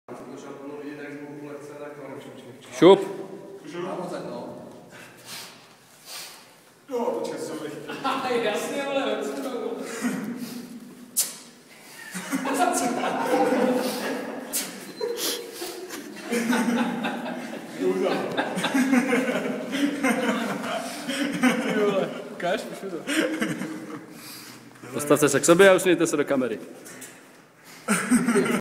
Human swings @ 75kg
Human TGU - to už jsme viděli! Ale co human swingy? :) Točeno na tréninku v Hardstyle Gymu Praha